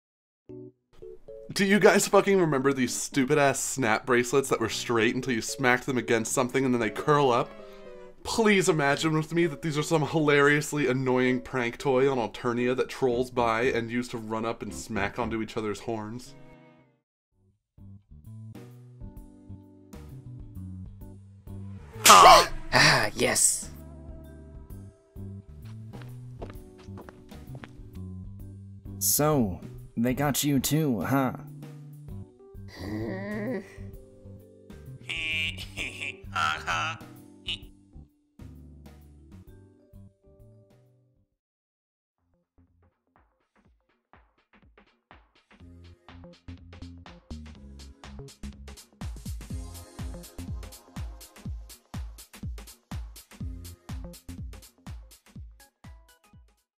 Snap Bracelets: VoFT Homestuck Comic Dub
Get pranked BRO!
Vriska: Crab
Eridan: Tech
Tavros: Devin
Gamzee: Press
Editor: Erik
Artist: We couldnt find the original artist. If you know please tell us.